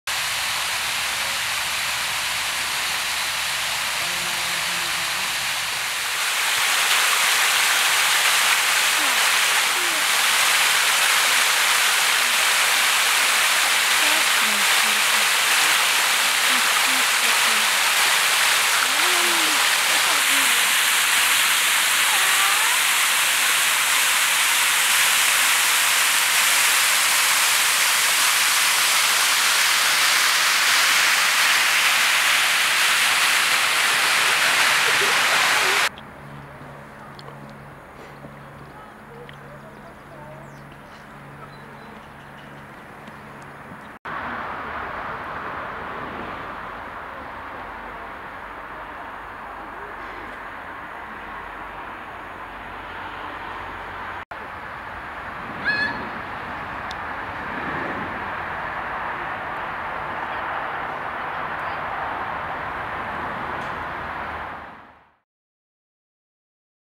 Waterval van de Vrijenberger Spreng ook wel de kleine Loenense waterval genoemd.
De Watervallen van de Vrijenberger Spreng zijn de grootste twee watervallen van Nederland en liggen nabij de Gelderse plaats Loenen.

De Vrijenbergse Spreng werd aan het einde van de 19e eeuw gegraven in opdracht van koning Willem 1, zodat het waterpeil van het Apeldoorns Kanaal op peil kon worden gehouden.
Vanwege het grote hoogteverschil in de spreng, werden er twee watervallen aangelegd. De grote waterval heeft een verval van 15 meter. Op deze video zie je de kleine waterval.